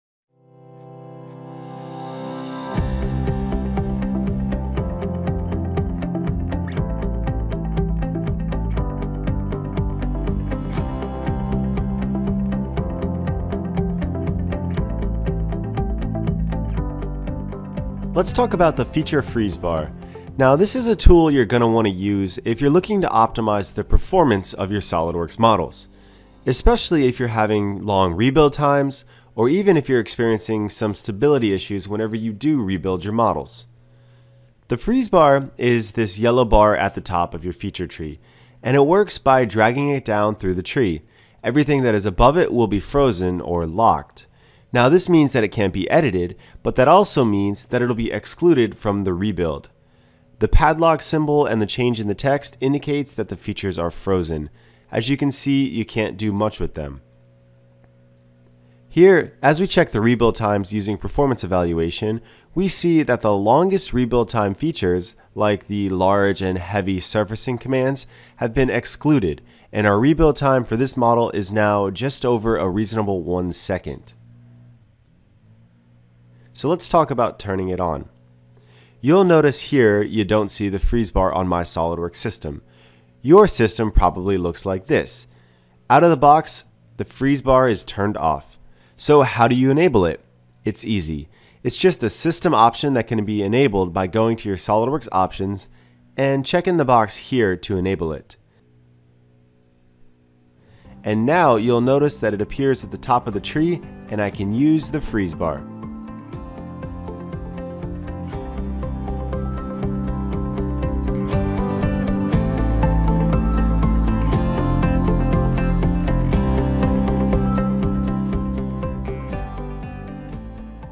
Video Tech Tip: Using Feature Freeze Bar In SOLIDWORKS
This is the tool you'll want to use if you're looking to optimize performance of your SOLIDWORKS models especially if you're having long rebuild times or if you're experiencing stability issues.

Join TriMech Application Engineer, Stephen Petrock, as he demonstrates the capabilities of this intuitive feature. To check out other Video Tech Tips, click this link to our weekly blog